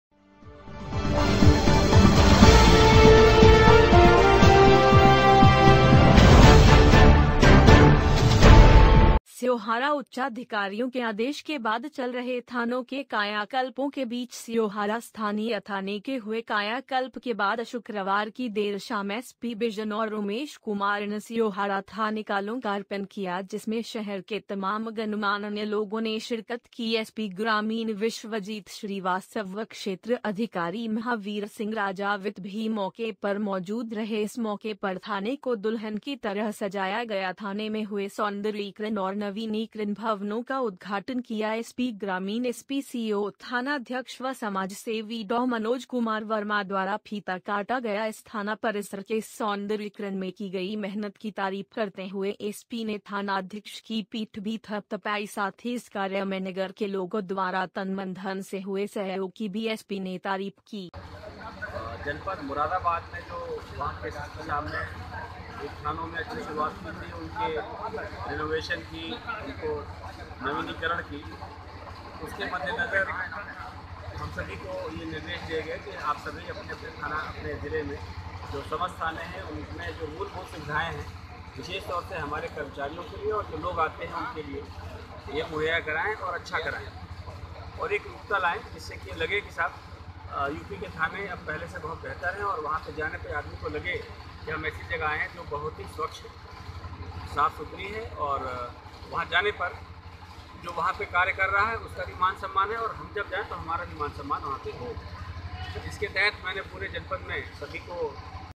ने किया स्योहारा थाने का
SKK NEWS एसपी उमेश कुमार सिंह ने किया स्योहारा थाने का लोकार्पण
संजय शर्मा की खास रिपोर्ट
स्योहारा उच्चाधिकारियों के आदेश के बाद चल रहे थानों के कायाकल्पो के बीच स्योहारा स्थानीय थाने के हुए कायाकल्प के बाद शुक्रवार की देर शाम एसपी बिजनौर उमेश कुमार ने स्योहारा थाने का लोकार्पण किया जिसमें शहर के तमाम गणमान्य लोगों ने शिरकत की एसपी ग्रामीण विश्वजीत श्रीवास्तव व क्षेत्र अधिकारी महावीर सिंह राजावत भी मौके पर मौजूद रहे इस मौके पर थाने को दुल्हन की तरह सजाया गया थाने में हुए सौंदर्यीकरण और नवीनीकरण भवनों का उद्घाटन किया एसपी ग्रामीण एसपी सीओ थानाध्यक्ष व समाज सेवी डॉ मनोज कुमार वर्मा द्वारा फीता काटा गया इस  थाना परिसर के इस सौन्दर्यकरण  मे की गई मेहनत की तारीफ करते हुए एसपी ने थानाध्यक्ष की पीठ भी थपथपाई साथ ही इस कार्य मे नगर के लोगो द्वारा तन,मन धन से हुए सहयोग की भी एसपी ने तारीफ की
इस मौके पर तमाम अधिकारियों के साथ साथ थाने के  स्टाफ में मौजूद एसआई रविंद्र कुमार नैन, सुधीर तोमर, सुरेंद्र सिंह,राजेंद्र शर्मा, भी मौजूद रहे तो नगर से व्यापारी नेता अरुण कुमार वर्मा,चौधरी फहीम उर रहमान, डॉ एचएस कॉलरा, असजद चौधरी,अलीमुद्दीन अंसारी, आशीष माहेश्वरी, आदि भी मौजूद रहे